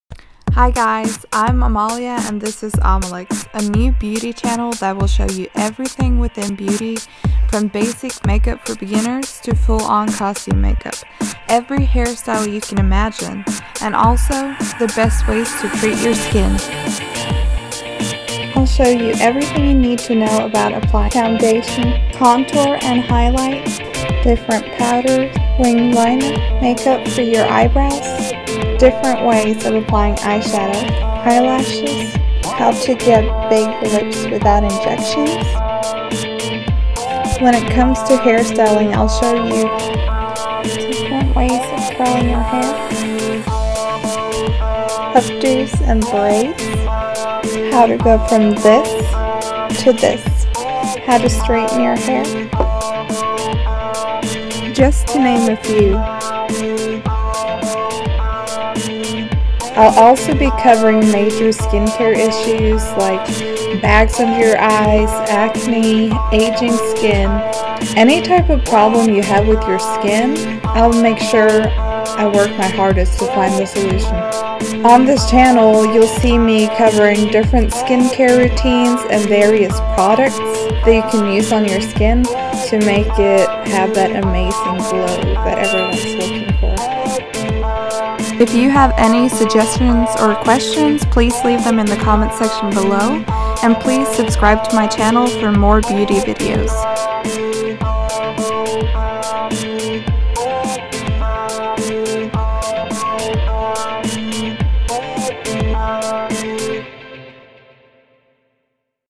Amalix Beauty Tutorials - Channel Trailer
Welcome to Amalix, a new beauty channel that will cover all aspects of Makeup, Hair and Overall Well-Being (including skincare). Sounds interesting?

This is my first video on YouTube, and I am a little bit shy, so please be kind. If you have any suggestions, or ideas for my channel, please let me know through Social Media, E-mail, or simply the comment section!